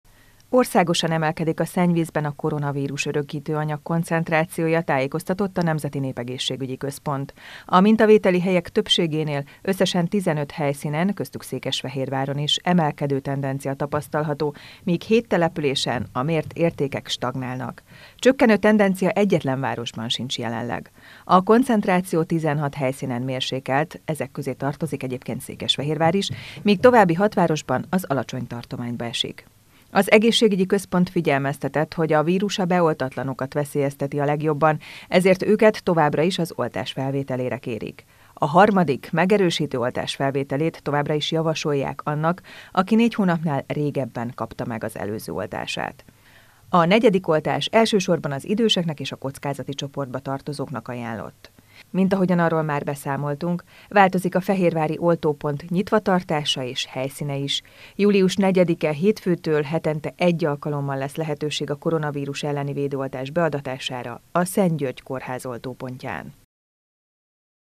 ROMLANAK AZ ADATOK
Ismét romlanak az adatok, Székesfehérváron is emelkedett a szennyvíz koronavírus koncentrációja. Csökkenő tendencia egyetlen városban sem tapasztalható.